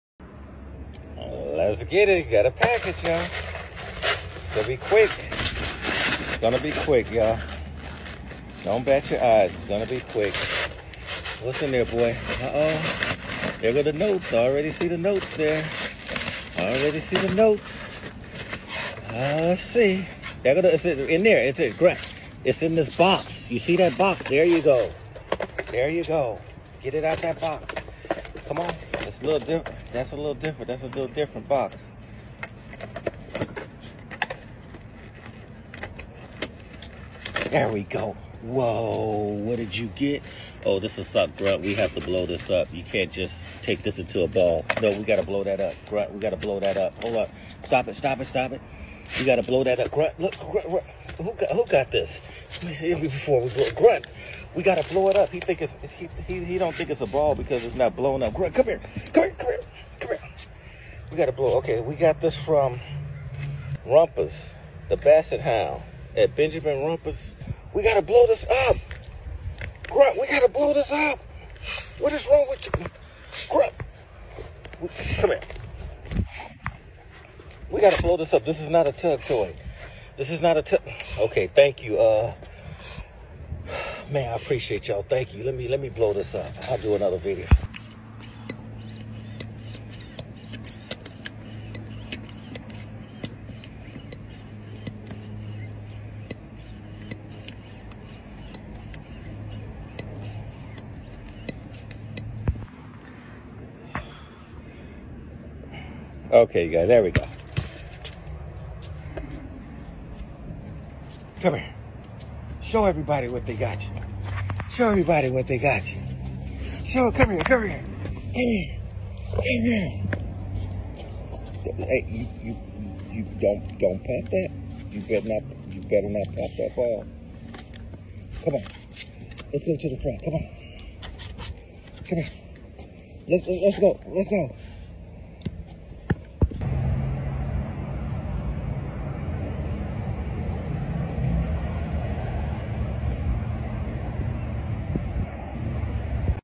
HUGE DOG DOES HIS OWN UNBOXING VIDEOS!
He don’t need people to open his toys